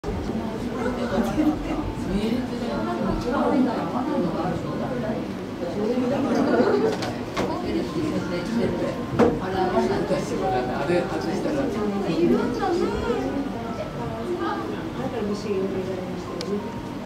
ユウレイクラゲ クラゲ解説コーナー 山形の加茂水族館へ行って来ました～♪ C0002
ユウレイクラゲ
クラゲ解説コーナー
山形の加茂水族館へ行って来ました～♪
2019年5月18日